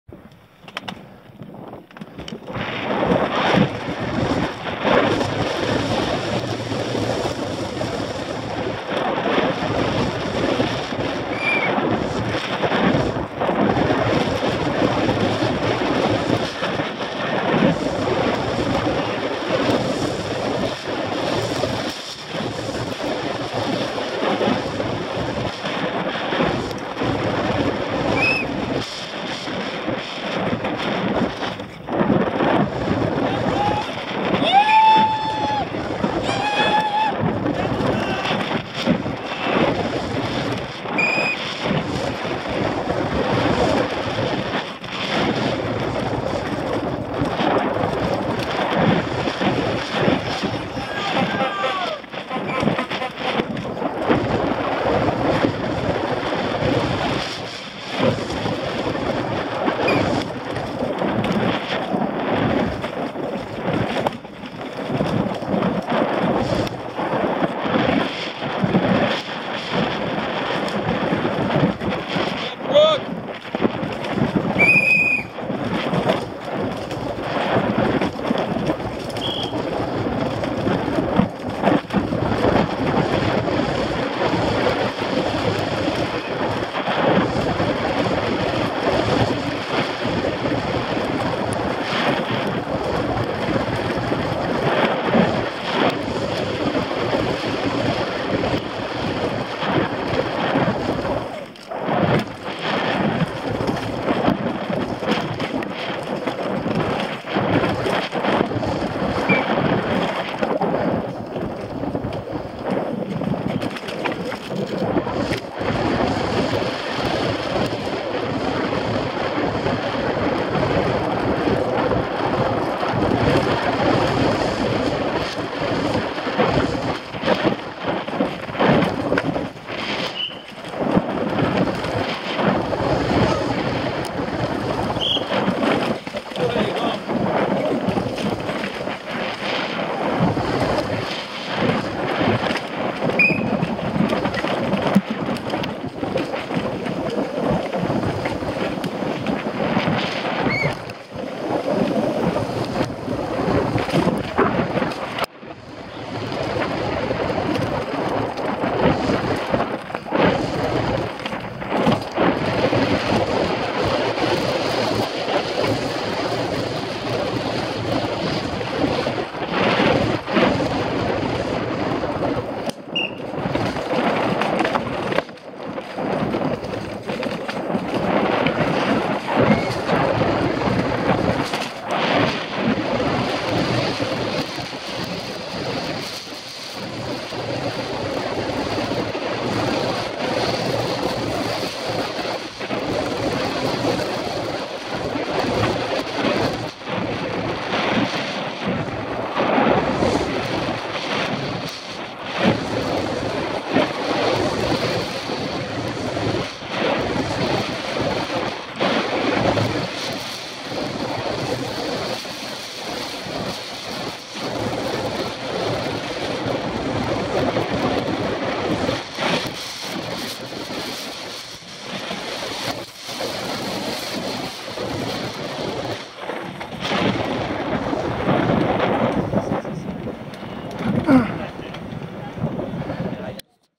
Meribel 2014 helmet camera with Brook Macdonald
Take a run down the course of the 2014 World Cup Downhill final in Meribel with Brook Macdonald.